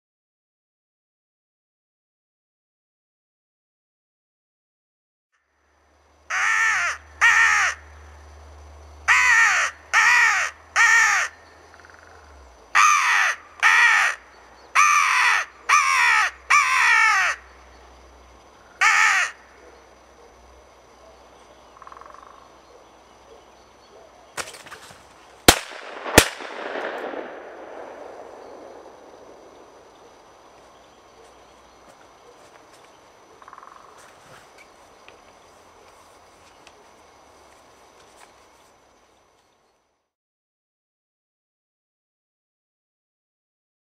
Krage, Due, gåseskjul
Se hvor hurtigt du kan åbne og skyde fra dit halmballeskjul.